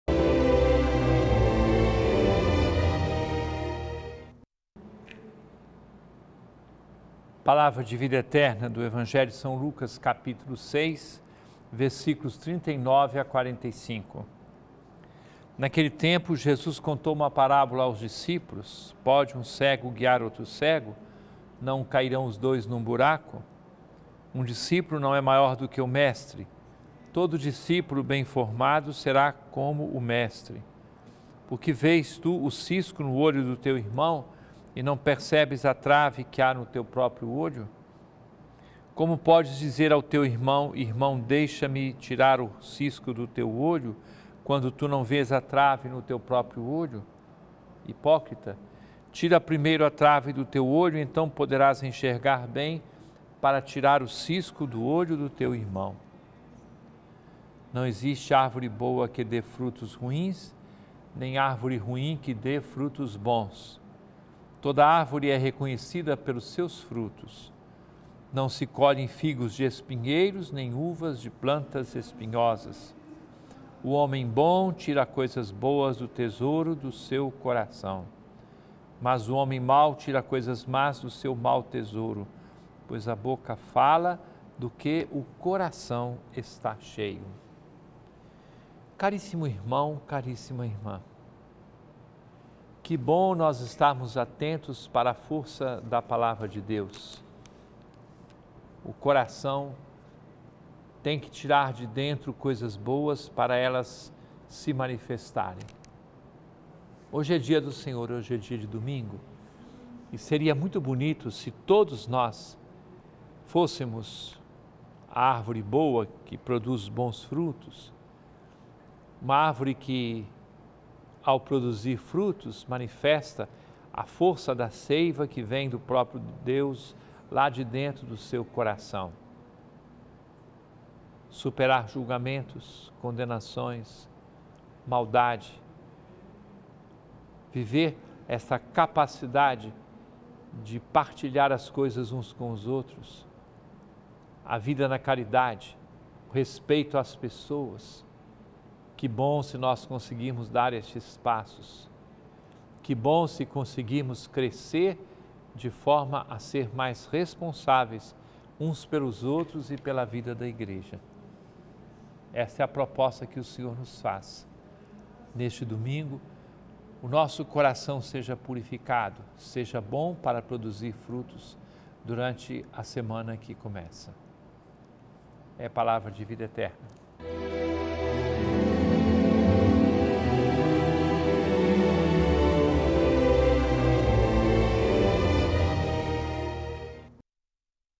Palavra de Vida Eterna | São Lucas 6,39-45 - 03/03/2019
Naquele tempo, 39Jesus contou uma parábola aos discípulos: “Pode um cego guiar outro cego? Não cairão os dois num buraco?

40Um discípulo não é maior do que o mestre; todo discípulo bem formado será como o mestre.

41Por que vês o cisco que está no olho do teu irmão, e não percebes a trave que há no teu próprio olho?

42Como podes dizer a teu irmão: ‘Irmão, deixa-me tirar o cisco do teu olho’, quando não percebes a trave no teu próprio olho? Hipócrita! Tira primeiro a trave do teu olho, e então poderás enxergar bem para tirar o cisco do olho do teu irmão.

43Não existe árvore boa que dê frutos ruins, nem árvore ruim que dê frutos bons.44Toda árvore é reconhecida pelos seus frutos. Não se colhem figos de espinheiros, nem uvas de plantas espinhosas.

45O homem bom tira coisas boas do bom tesouro do seu coração. Mas o homem mau tira coisas más do seu mau tesouro, pois sua boca fala do que o coração está cheio”.